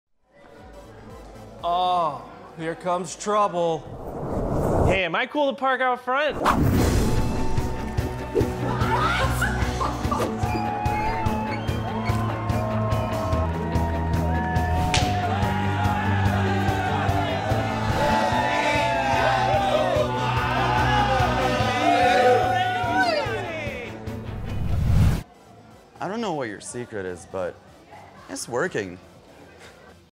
OLE SMOKY MOONSHINE| HOUSE OF OLE SMOKY | THE HOUSE PARTY
An average Joe transforms a little get together into a wild party when he shows up with a jar of Apple Pie.